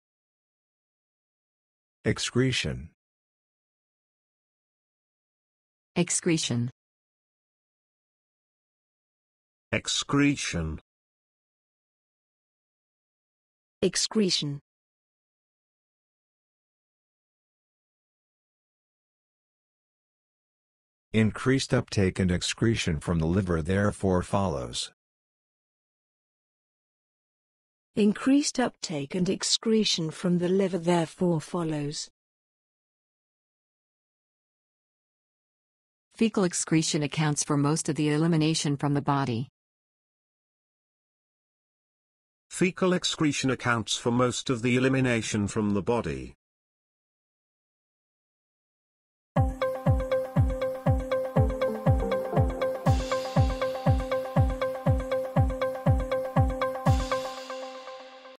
EXCRETION pronunciation | Improve your language with bab.la
Improve your spoken English by listening to EXCRETION pronounced by different speakers – and in example sentences too. Learn and love languages with bab.la.